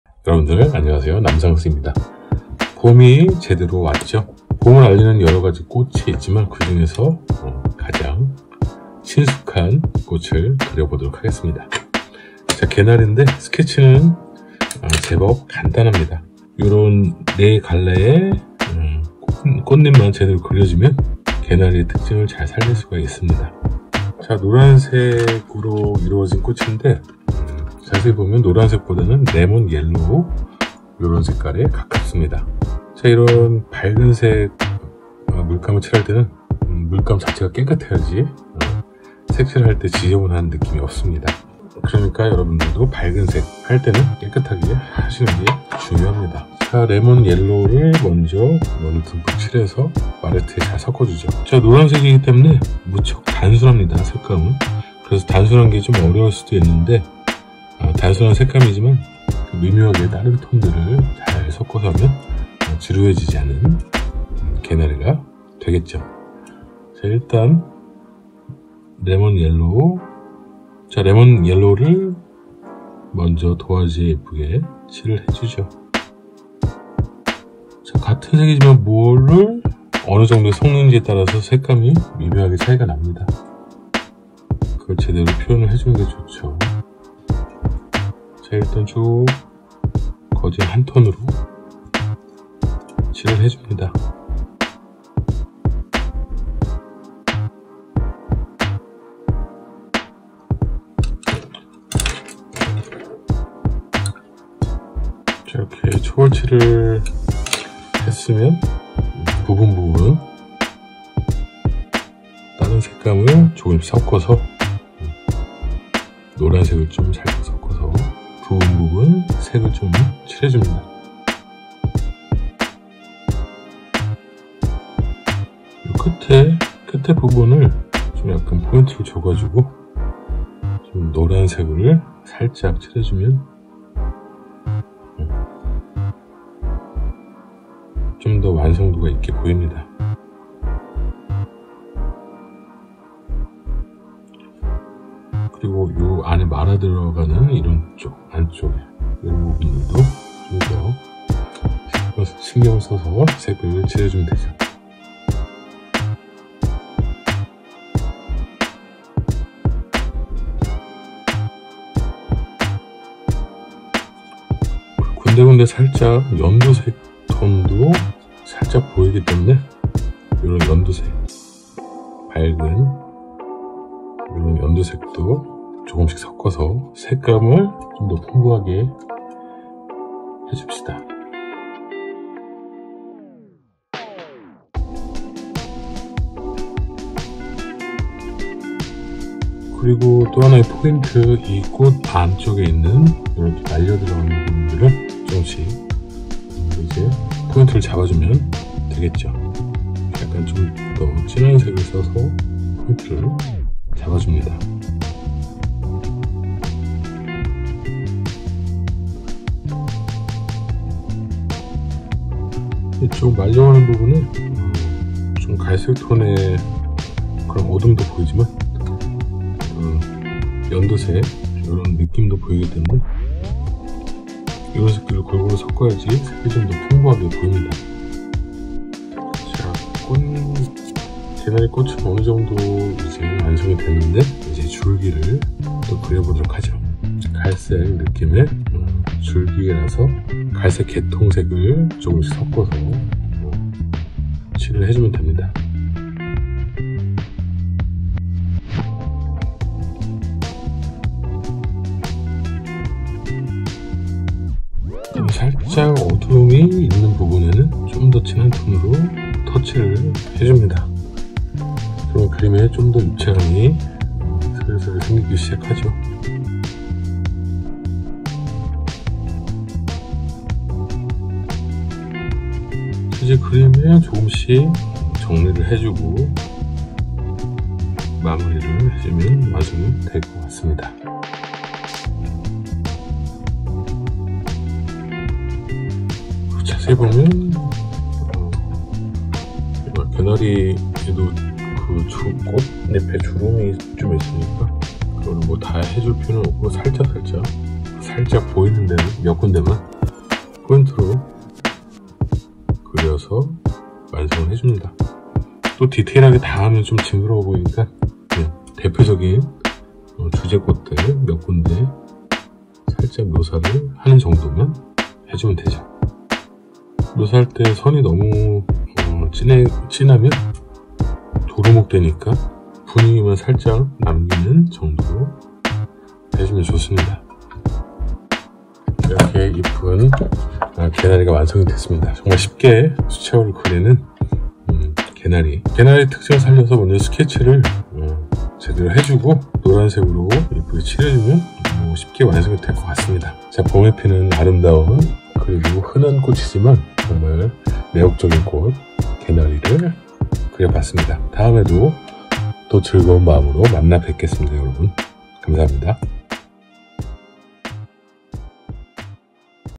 봄의 상징 개나리 ~ 수채화로 쉽게 그리는 개나리꽃 Easy watercolor tutorial Forsythia
봄을 알리는 꽃,
흔하게 보이는 꽃이지만, 사랑스러운 개나리
아기자기한 개나리꽃을  수채화로 쉽게 그려봅니다.
즐거운 시청 바랍니다!~